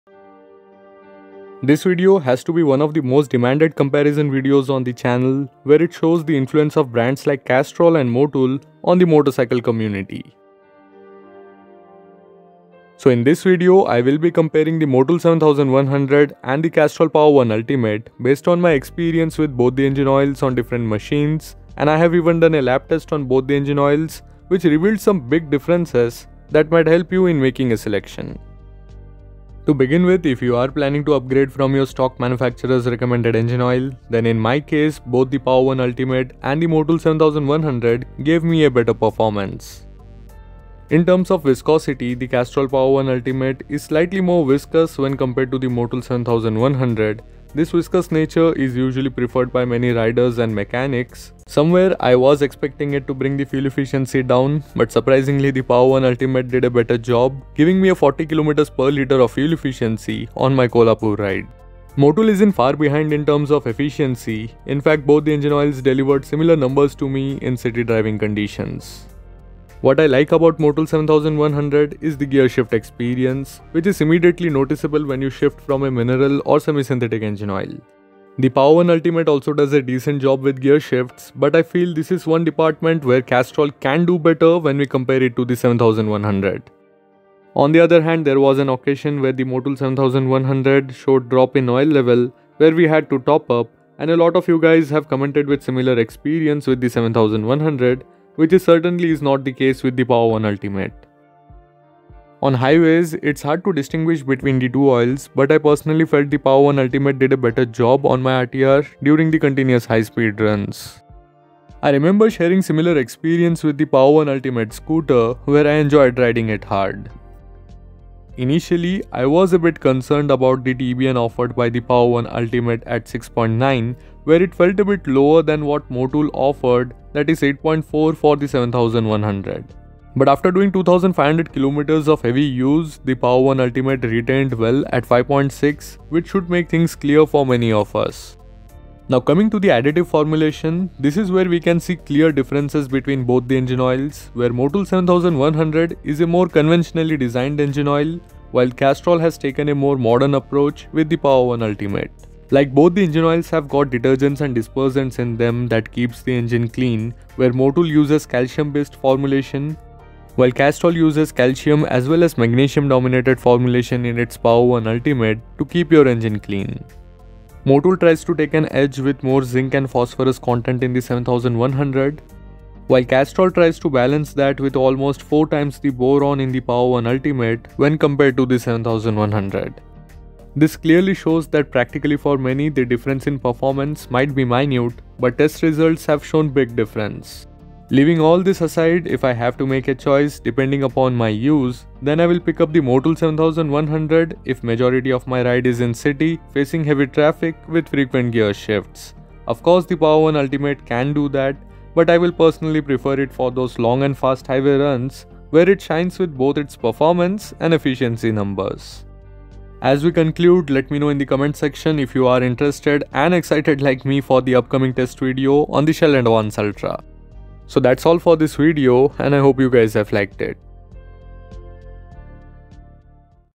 MOTUL 7100 VS CASTROL POWER1 ULTIMATE REVIEW BEST SYNTHETIC ENGINE OIL FOR BIKES CITY & HIGHWAY RIDE
Best motorcycle engine oil? Best fully synthetic engine oil for bikes? Motul 7100 vs Castrol Power1 ultimate?
In this video we will compare the popular Motul 7100 and Castrol Power1 Ultimate engine oil. I have used and experienced both the Castrol power1 ultimate and Motul 7100 on different motorcycle and Scooters like Bajaj Dominar, TVS Apache, Honda Unicorn, Honda Dream Yuga, Honda Activa, TVS Jupiter etc. This video compares both the Motul 7100 and Castrol Power1 ultimate right from their viscosity, TBN, highway and city performance to their formulation. The additive formulation reveals a lot about both Motul 7100 and Castrol Power1 ultimate where the comparison can be made on their cleaning agents, anti wear and anti corrosion agents along with other significant additives present in Motul 7100 and Castrol Power1 ultimate.

Motul 7100

Castrol Power1 ultimate

Castrol Power1 ultimate 20W-50

Tool kit 1

Tool kit 2